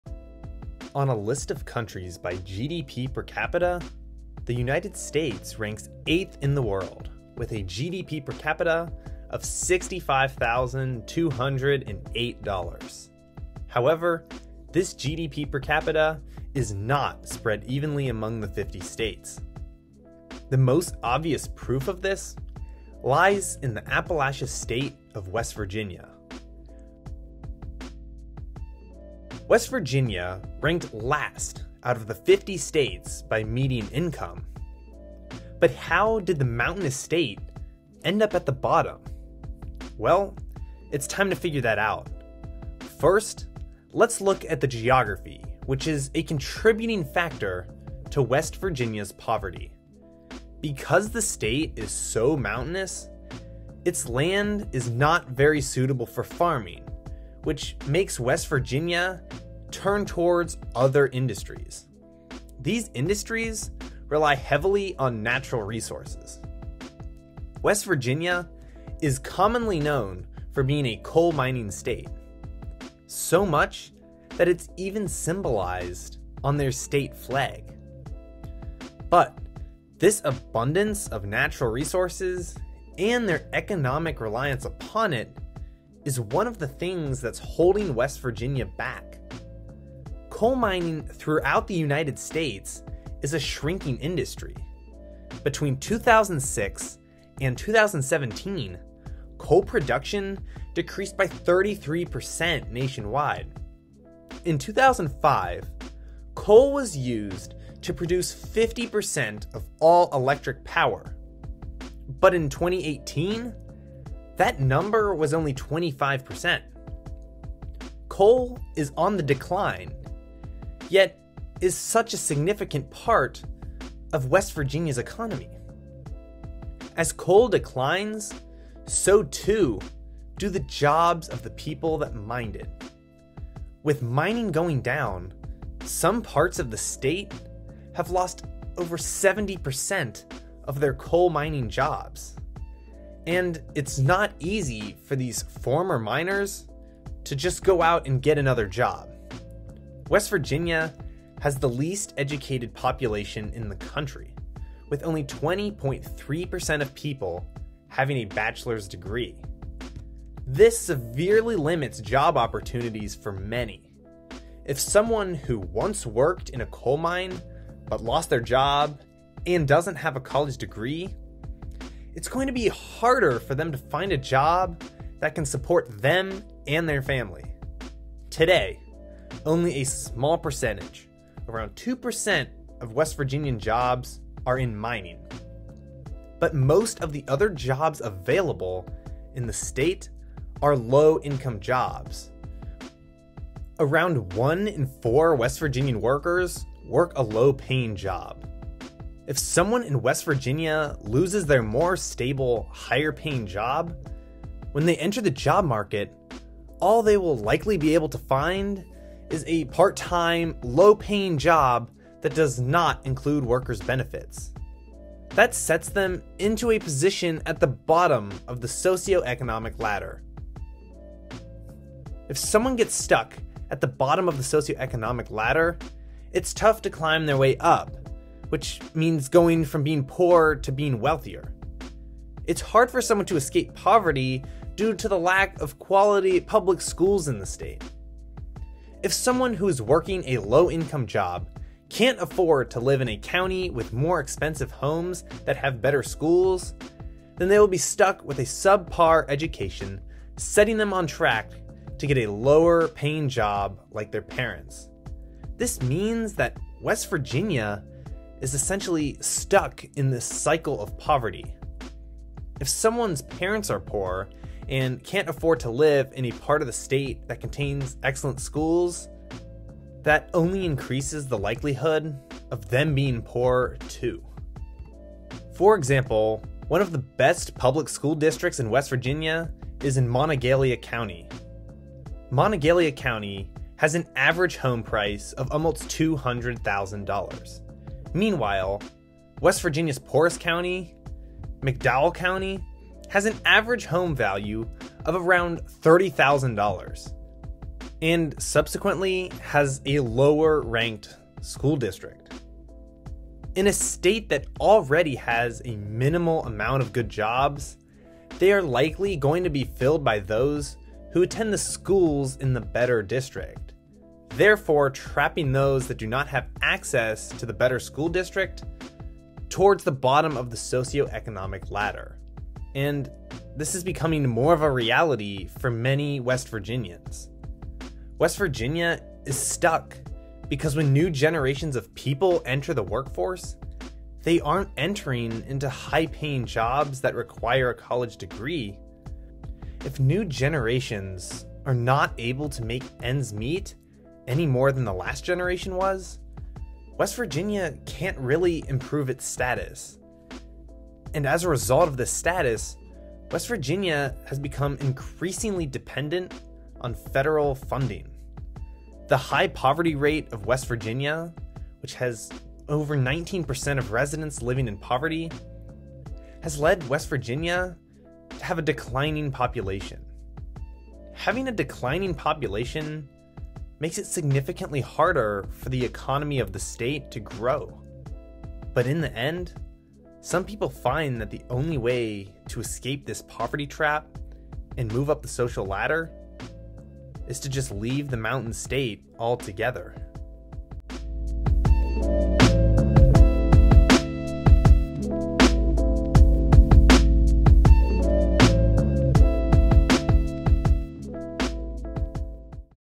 How West Virginia became the Poorest State [Documentary]
The State of West Virginia has the Appalachian mountains and beautiful scenery.  However, when it comes to wealth, education, and jobs, West Virginia falls behind the other 49 US states.